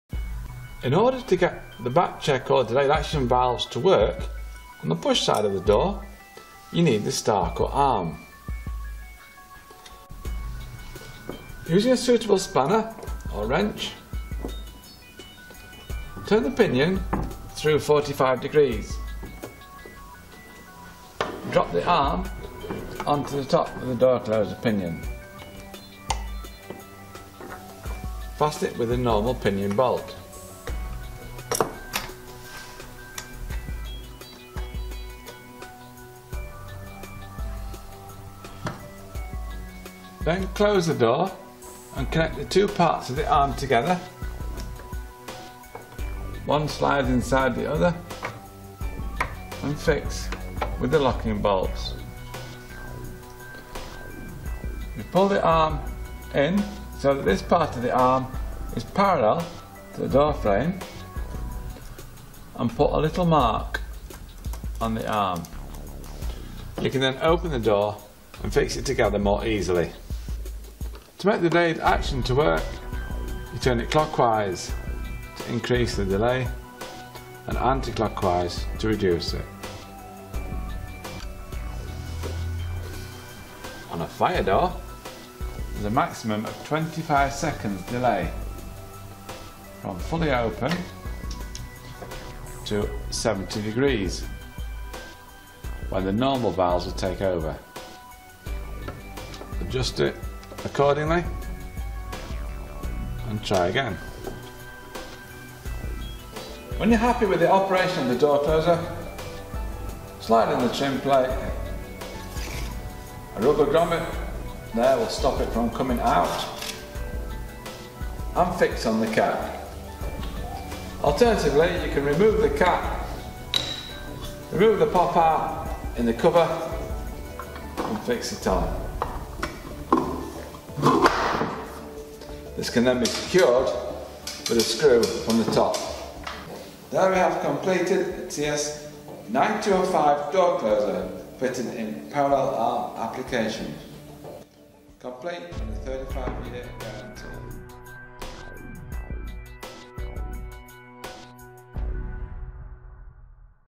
Rutland: Know How - Rutland TS.9205 How to adjust - Delayed Action
How to adjust the delayed action valve on the Rutland TS.9205 door closer  when fitting on the push side figure 66 of the door.
The variable spring EN2-EN5 door closer is suitable for doors up to 100kg/1250mm wide.